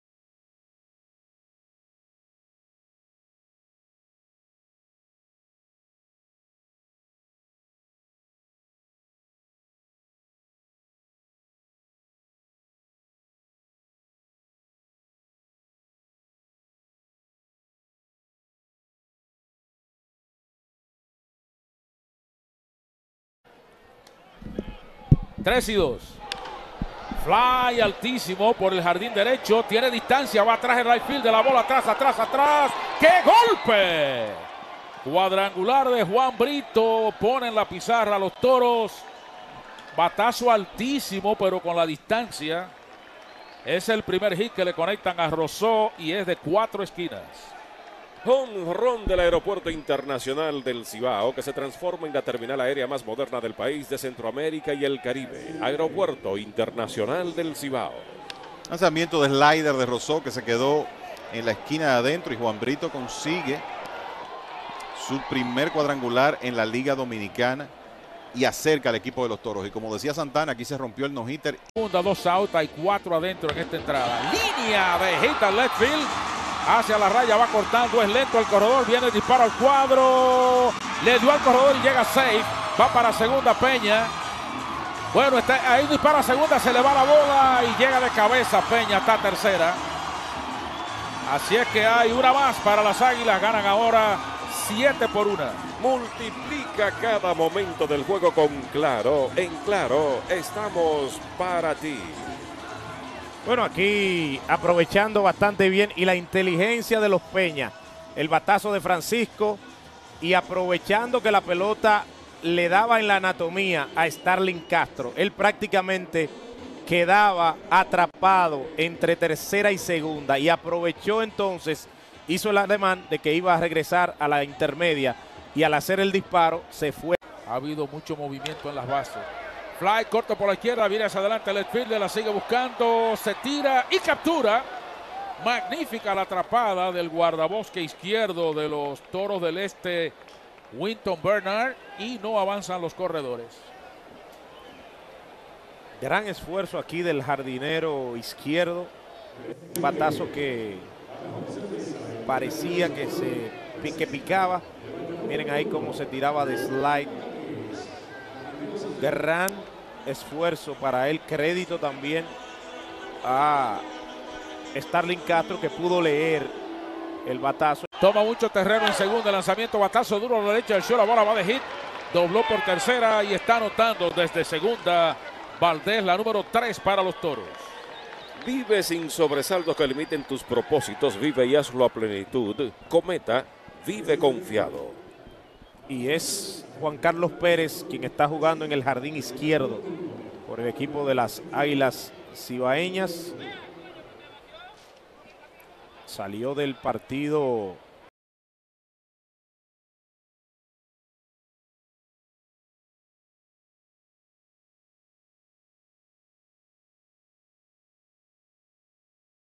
RESUMEN TOROS DEL ESTE VS ÁGUILAS CIBAEÑAS | 21 OCT 2023 | SERIE REGULAR LIDOM
¿NUEVOS EQUIPOS EN EL INVIERNO? EL ESCOGIDO EXPLICA EN DETALLE CÓMO VE A LA LIGA INVERNAL LIDOM